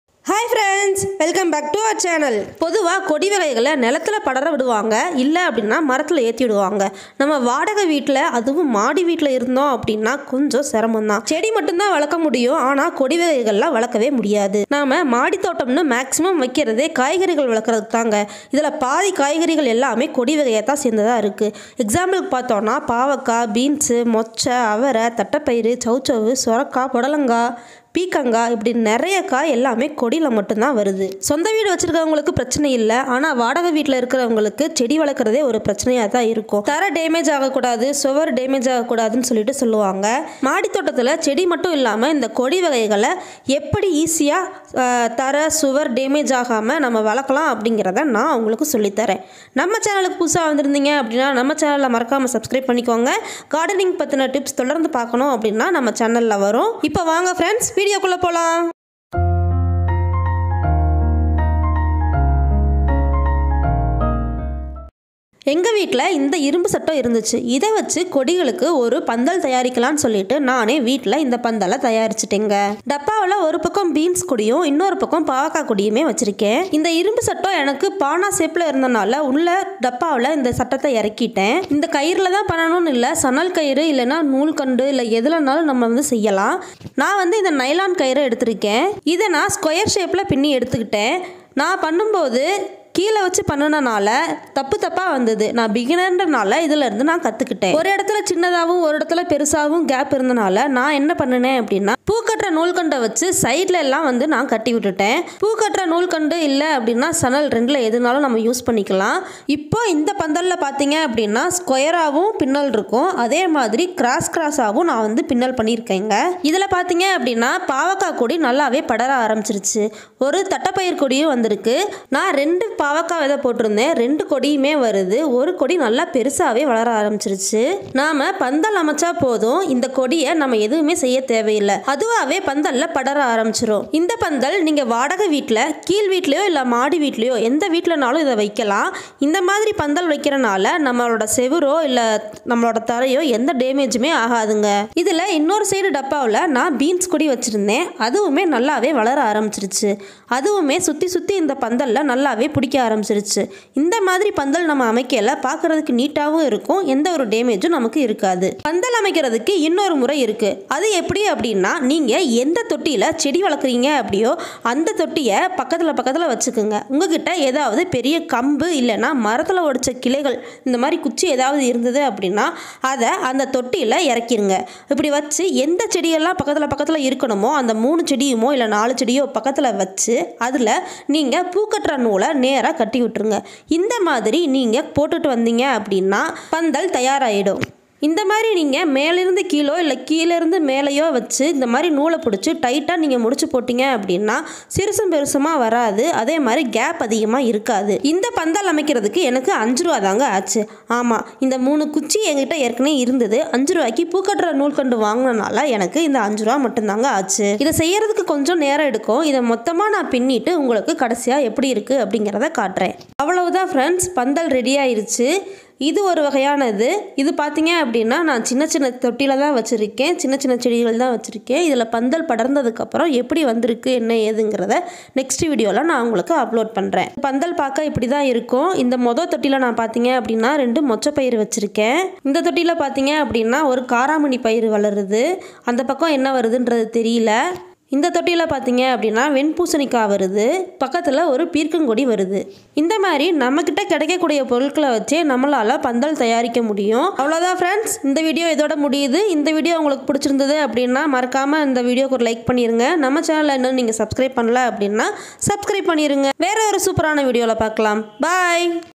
💥 எளிமையாக மாடி தோட்டத்தில் கொடி படற பந்தல் அமைப்பது எப்படி?😍| Simple Pandhal Idea | Terrace Garden
Hopefully everyone enjoy our videos.

‼️‼️‼️‼️Neela Nila Vlogs ‼️‼️‼️

Thanks for Supporting &Love.❤️🔥🔥